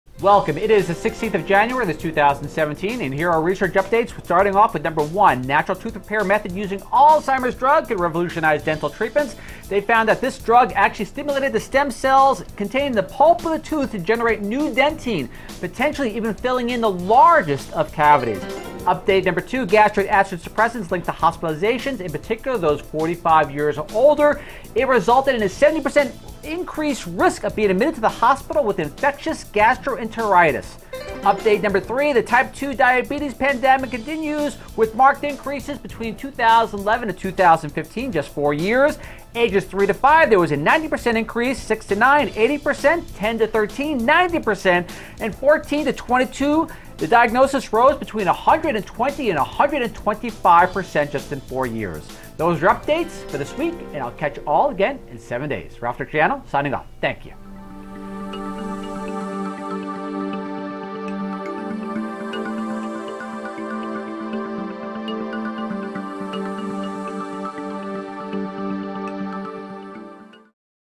📡 Breakthrough Research - 248
1. Natural tooth repair method, using Alzheimer's drug, causes dentine to regenerate
2. Gastric acid suppressants linked to a 70% increase in hospitalization for infectious gastroenteritis
3. Type 2 diabetes doubles in just 4 years among those 22 and younger